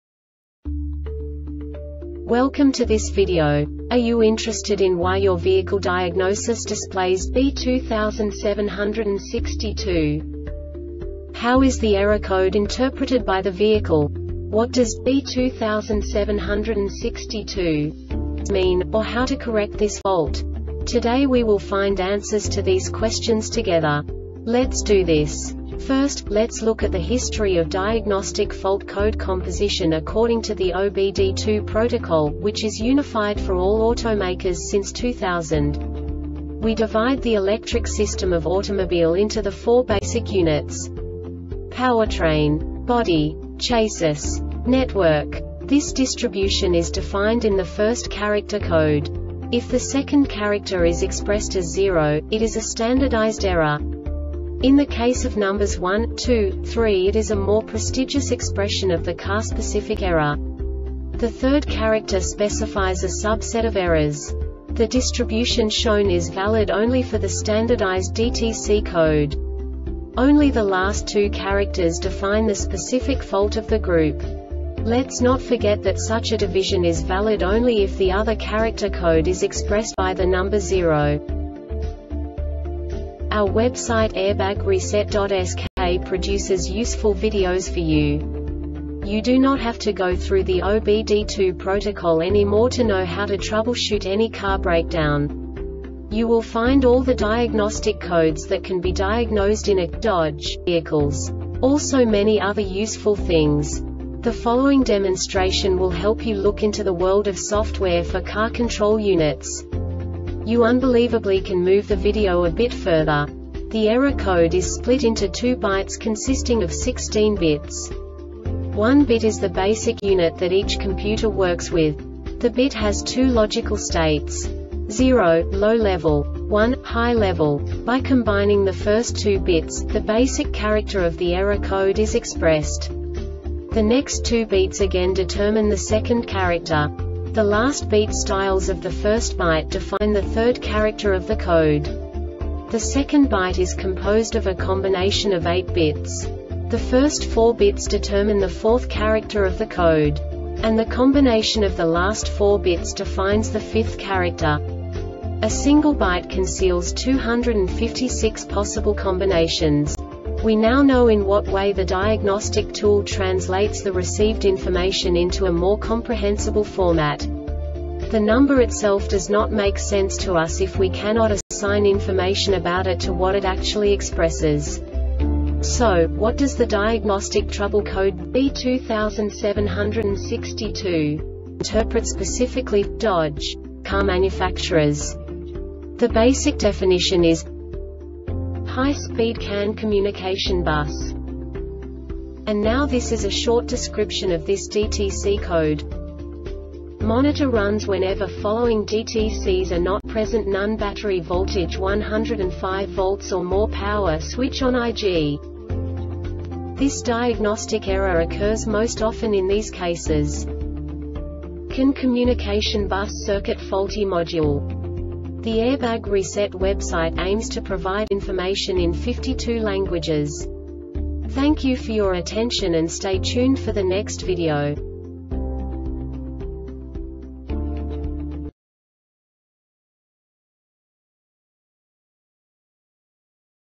DTC Dodge B2762 Short Explanation
Contents:
0:21 Basic DTC analysis according to OBD2 protocol standard.
1:48 Insight into programming
2:58 A diagnostically understandable description of the Diagnostic Trouble Code
3:05 Basic error definition

Make:
Dodge
Code:
B2762
Definition:
High Speed CAN Communication Bus
Description: